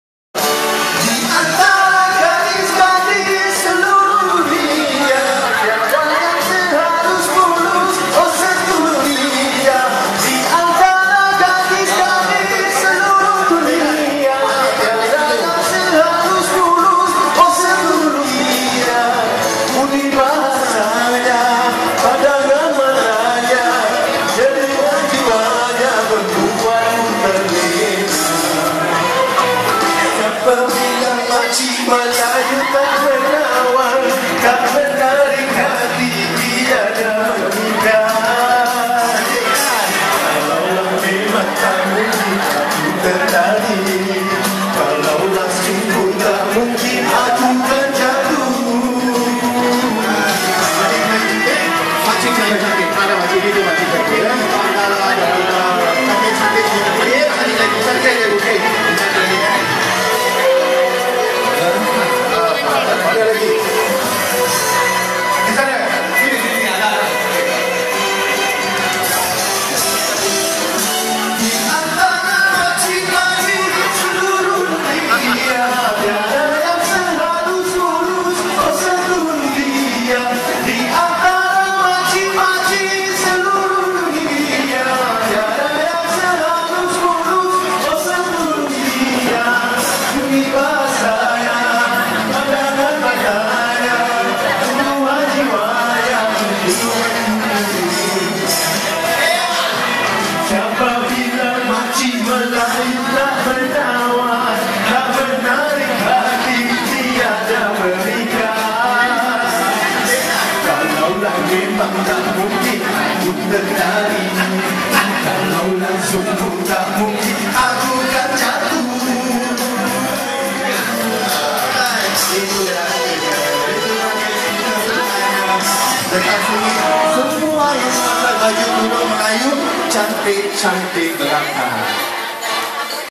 Gadis Melayu ke Makcik Melayu
Gadis Melayu Cantik dan Makcik Melayu pon cantik di majlis perkahwinan Riti & Effendi - 10 Nov 13. Hosted by Glamz Gallery.